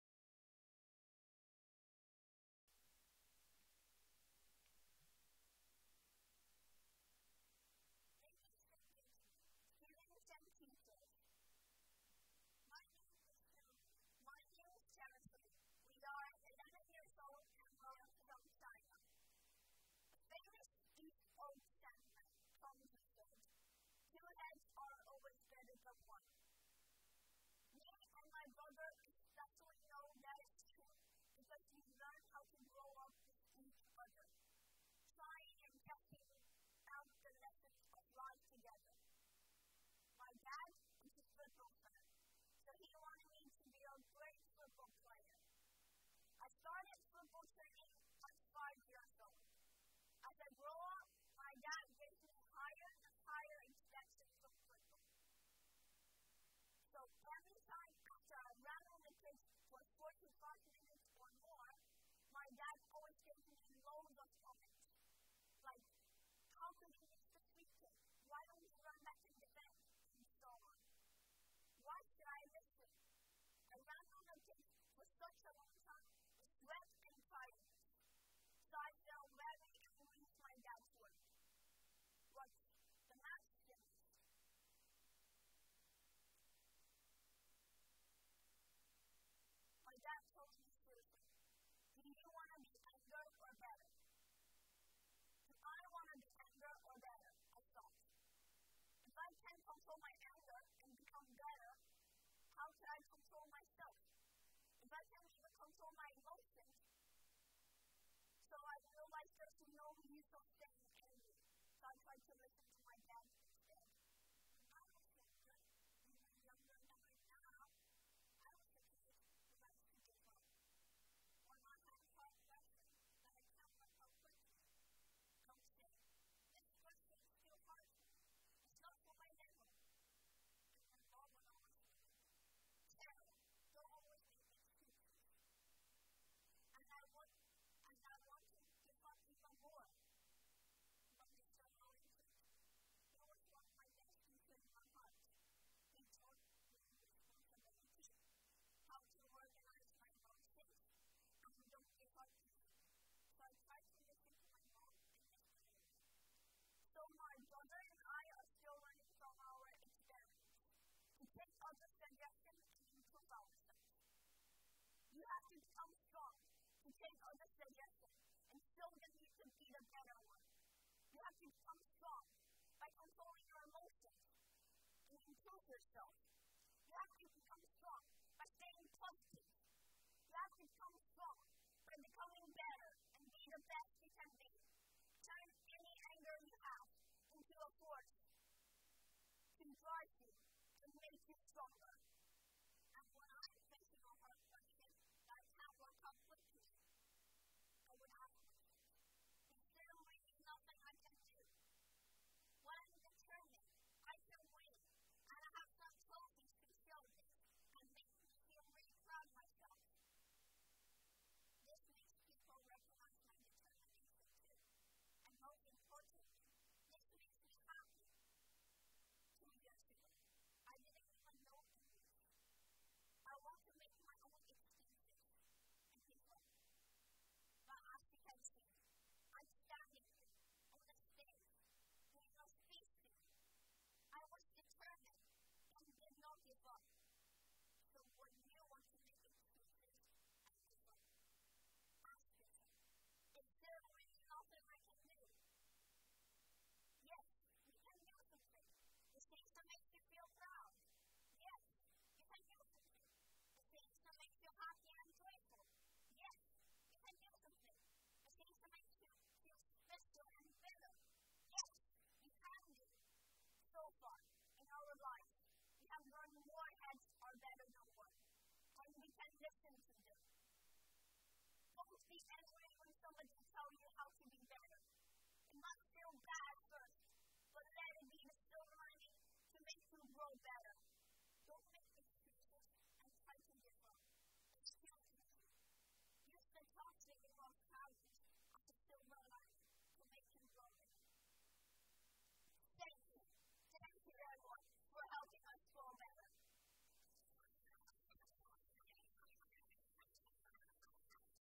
Two Heads Are Better Than One | Tianze Li and Tianyi Li | TEDxYouth@WIS
Anger and hardship can be problematic and sometimes overwhelming for an individual. Taking criticism from another person is equally as hard. Twin brothers, Tianze and Tianyi, explore how to deal with these emotions through working together to bring happiness and a joyful atmosphere to a period of distress through their life stories together. Their young minds explore how they learned together and the importance of looking past spiteful emotions.